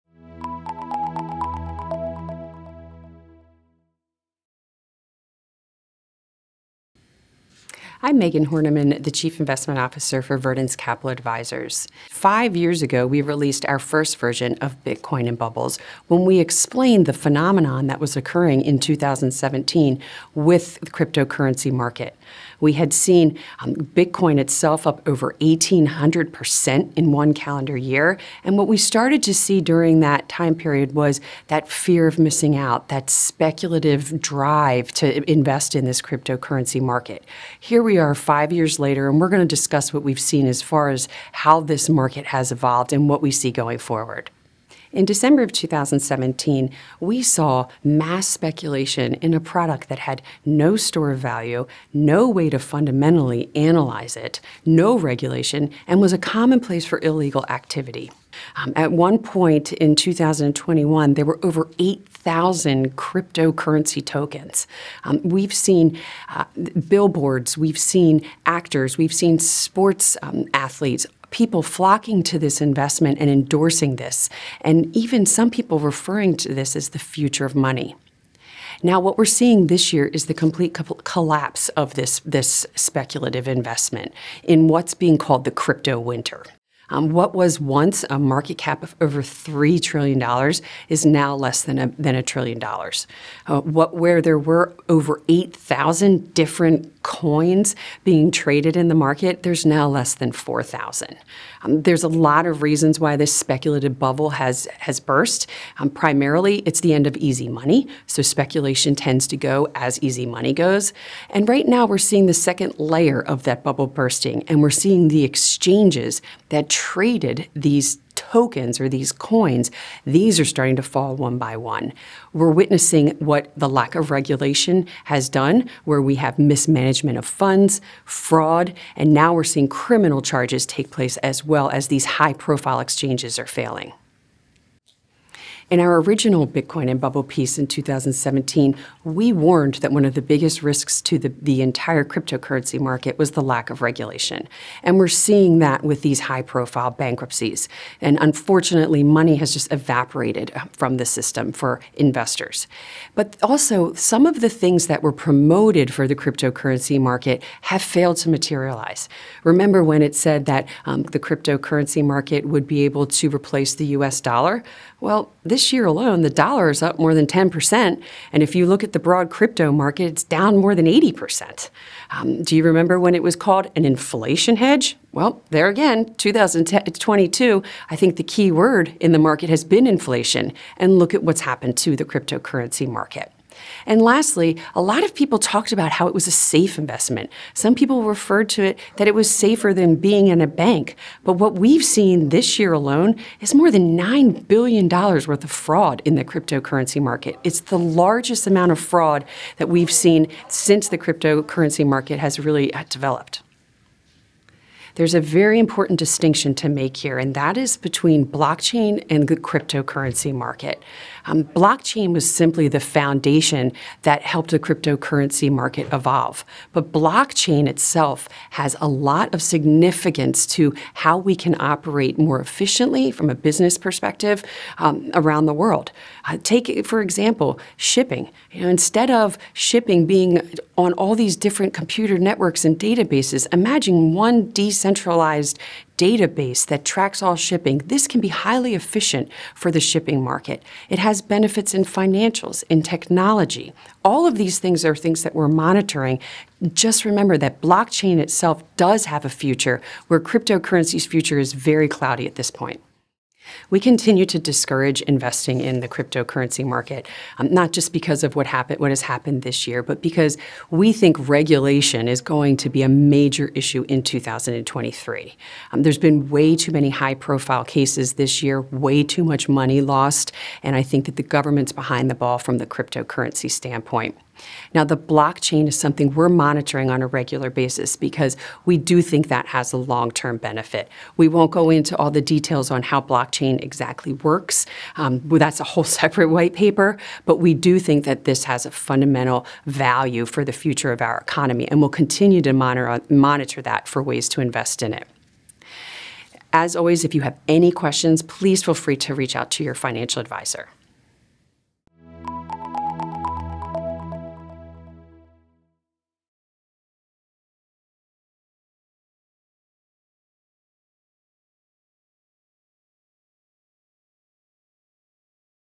4Q22 White Paper: Bitcoin Part 3
In this video, 🎥 Megan Horneman, the Chief Investment Officer for Verdence Capital Advisors, reflects on the evolution of the cryptocurrency market over the past five years. In 2017, Bitcoin saw a staggering 1800% increase in value, driven by the fear of missing out and speculative investment. However, as of 2023, the crypto market has undergone significant changes. 💰💥

Back in December 2017, the crypto market was characterized by mass speculation, lacking a store of value, regulatory oversight, and being a breeding ground for illegal activities. 🤯📉 At its peak in 2021, there were over 8,000 different cryptocurrency tokens, with celebrities and athletes endorsing it as the future of money. 🚀🌟

Fast forward to today, and we are witnessing the collapse of this speculative bubble, commonly referred to as the "crypto winter." ☃️❄️ The market capitalization, once exceeding three trillion dollars, has now dropped to less than a trillion, with fewer than 4,000 coins remaining. 💔💰

Several factors have contributed to this bubble burst. Firstly, the end of easy money has deflated speculation. 💸💨 Secondly, unregulated exchanges are crumbling, revealing mismanagement, fraud, and even criminal charges. 🚓🤔 The lack of regulation, as warned in 2017, has become a glaring issue. 🚫🧐

Furthermore, the promises made about cryptocurrencies have not materialized. They were supposed to replace the US dollar, but the dollar is stronger than ever in comparison. 💵🔥 The market was touted as an inflation hedge, but it has suffered an 80% decline. 💥💹 Claims of safety compared to banks have been shattered by over nine billion dollars' worth of fraud this year. 💔💸

It's crucial to distinguish between blockchain technology and the cryptocurrency market. Blockchain has the potential to revolutionize various industries by providing a decentralized and efficient database. Shipping, finance, and technology are just some areas where blockchain can be transformative. However, the future of cryptocurrencies remains uncertain. 🌐🤖

Megan advises against investing in the cryptocurrency market due to the increasing regulatory scrutiny and numerous high-profile cases. The focus should instead be on monitoring blockchain technology, which holds long-term promise for our economy. 🚫💼

If you have any questions or need financial advice, don't hesitate to reach out to your financial advisor. 🤝💼